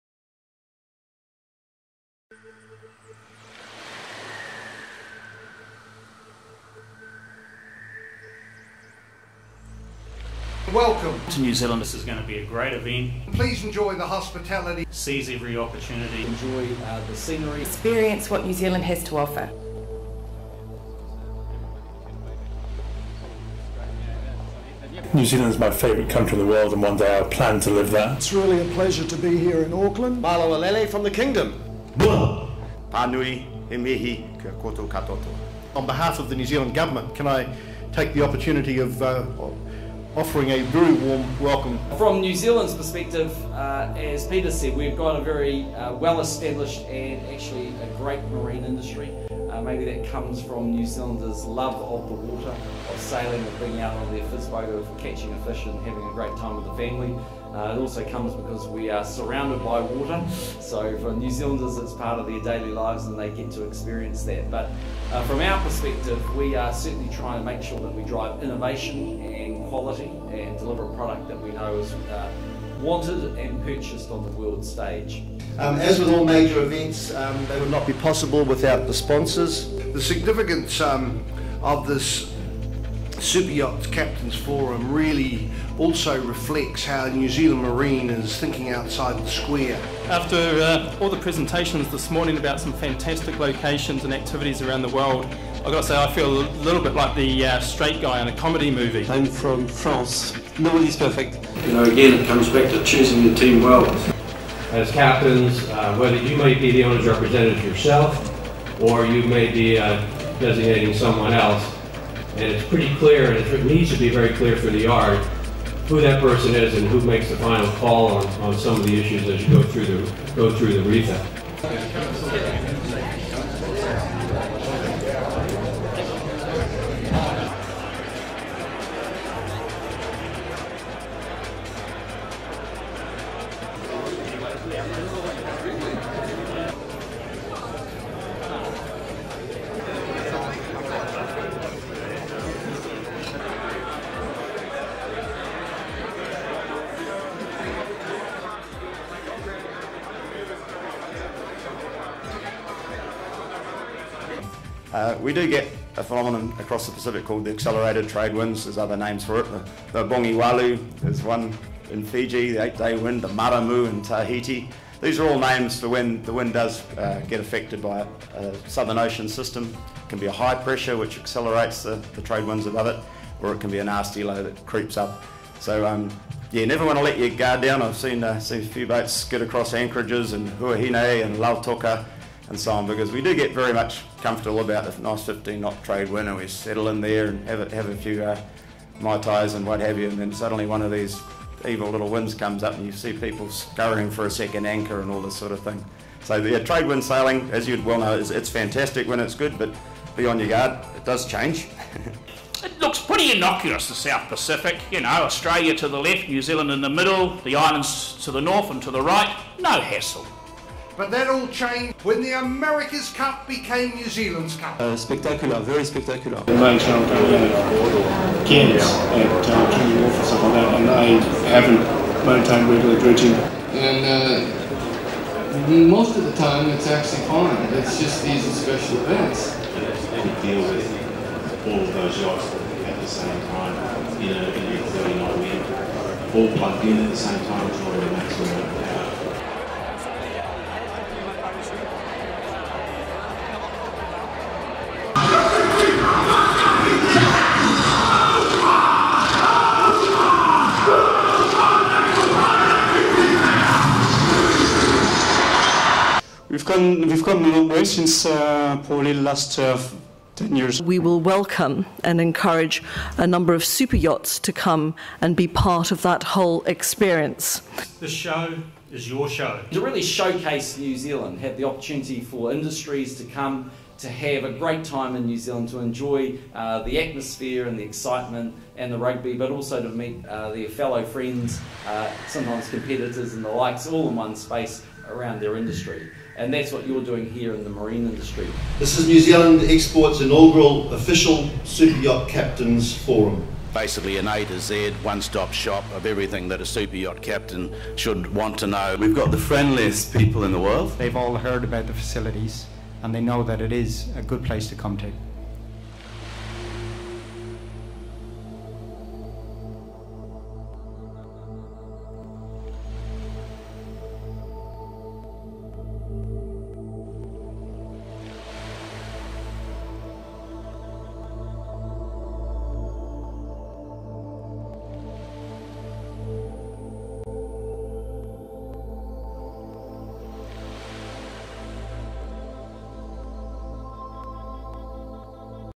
NZ Marine Superyacht Captains Forum 2011 - overview
Highlights from the inaugural NZ Marine Export Group Superyacht Captains Forum 2011, held at the Viaduct Events Centre, Auckland, September 15-16.